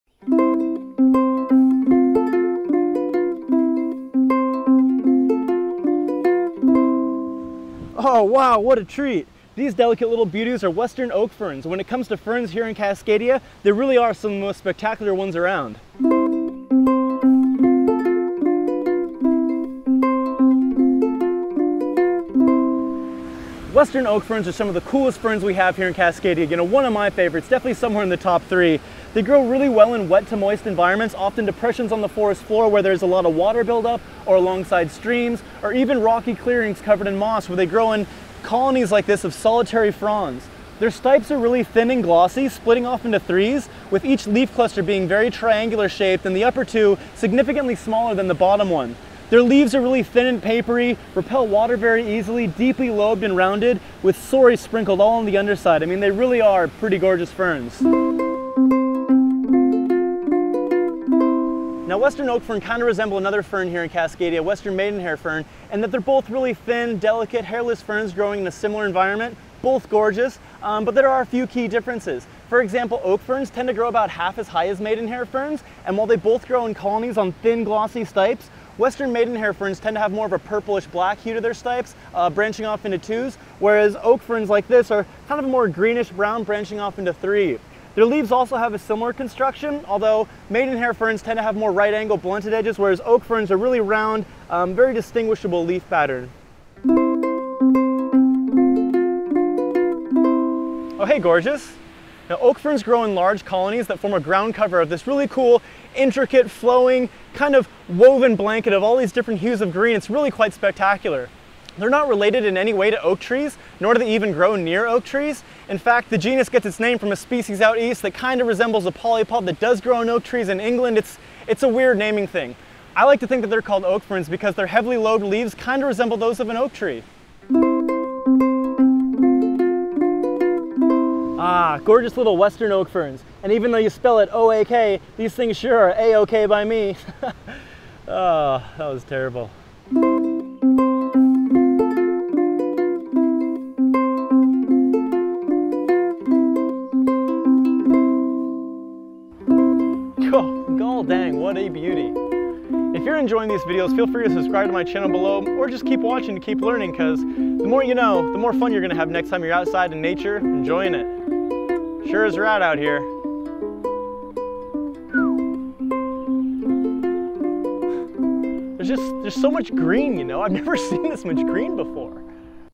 Oak Ferns - How to Identify Them! || Nerdy About Nature Flora Guide
One of the most delicate and gorgeous ferns we have here in Cascadia is the lovely little Western Oak Fern.  Characterized by their thin, glossy olive brown stipes that branch off into three leafs of delicate, papery, water-resistant leaflets, these little beauties tend to create a flowing, elegant ground cover in moist forest openings, scree fields or along streams.  They're definitely a sight to behold, keep an eye out for these next time you're out for a wander!

Like this vid?  Help support Nerdy About Nature to make more engaging videos like this possible!

Follow along for more engaging fun-facts to make your next jaunt into the outdoors more rad!

References:
- 'Plants of the Pacific Northwest Coast' by Jim Pojar & Andy MacKinnon.  Published by BC Ministry of Forests & Long Pine Publishing, Vancouver BC. 1994